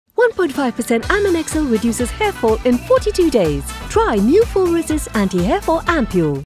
Regain Fuller Hair in 42 Days - NEW Full Resist Anti Hair Fall Ampoule
Get Fuller Hair

Video Description:
Tired of seeing your hair fall out?

Put a stop to it with NEW L'Oreal Paris Full Resist Anti Hair Fall Ampoule!

Packed with 1.5% Aminexil + Arginine to reduce hair fall in just 42 days.

Ready for stronger, fuller hair? Try now, because you're worth it.